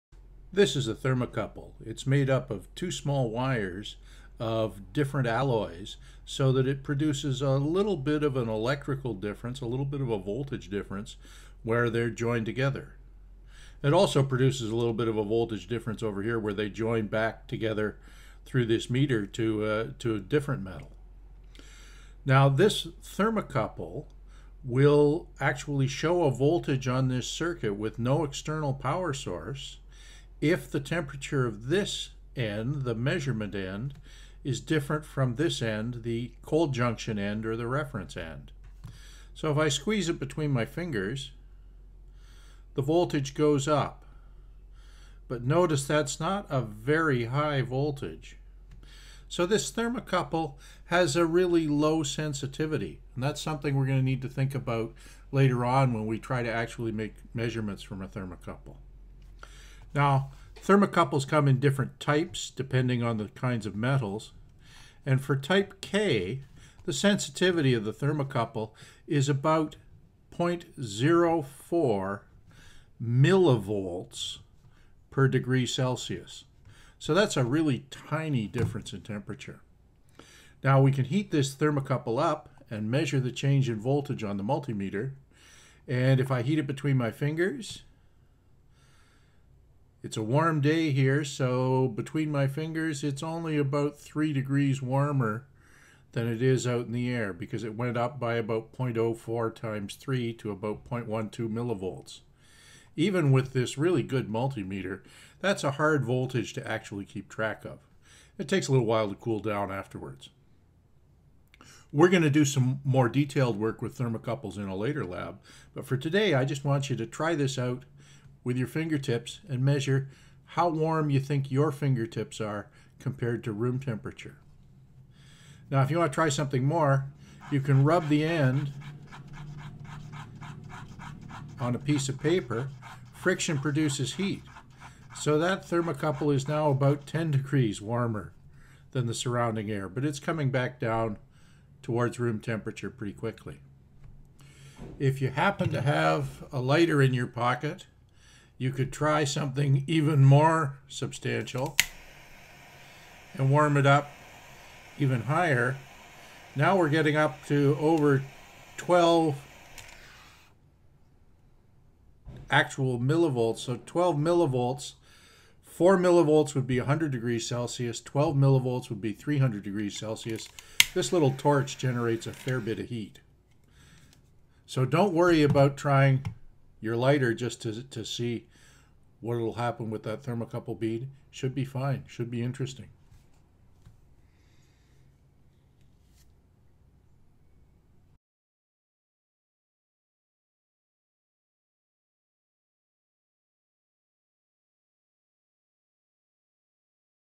Thermocouple Voltage
Type K thermocouples develop a voltage of around 4 mV per 100C, but much more when exposed to a flame.